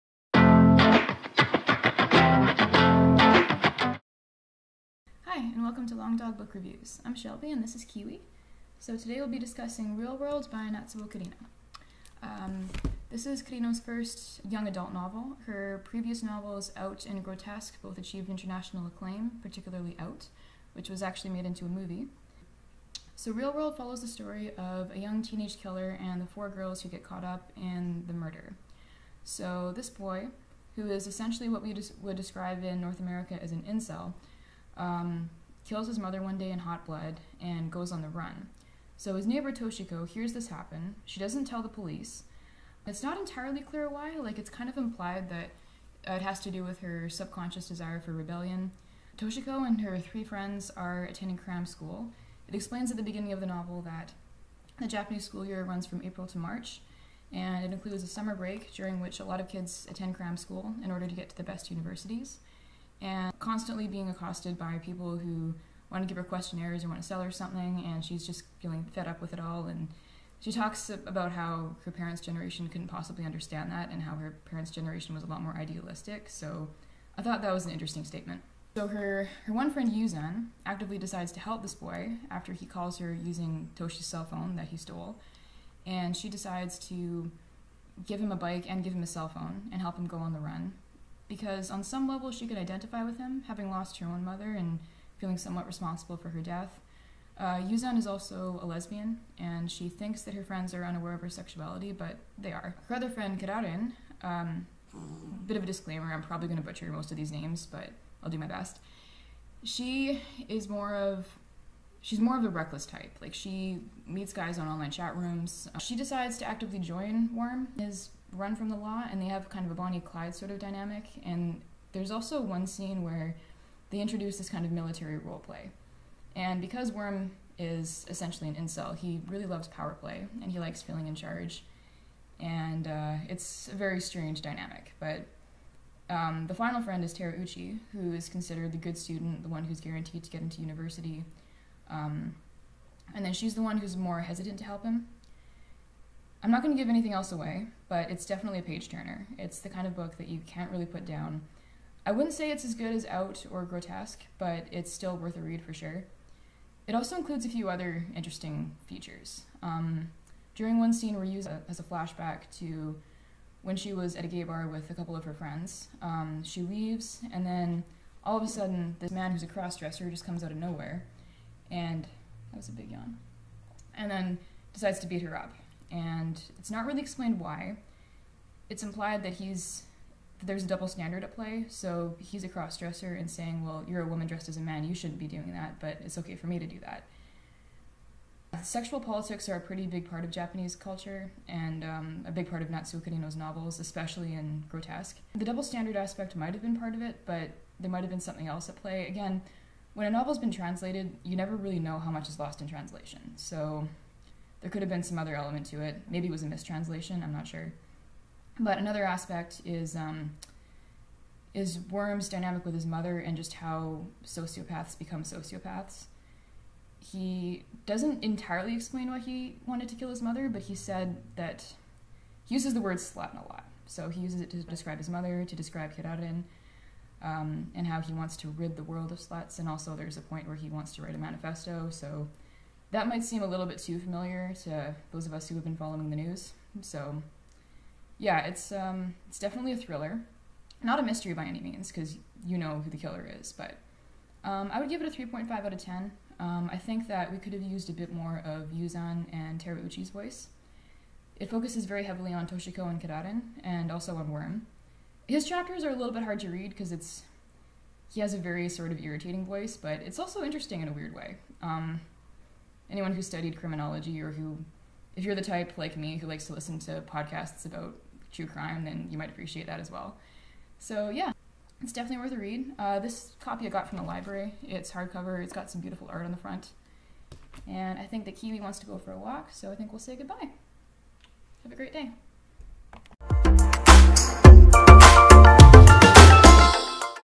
Review of 'Real World' by Natsuo Kirino
My first video book review! An author of feminist noir fiction writes a young adult novel. *I'm aware that the audio is not great. This is being fixed*.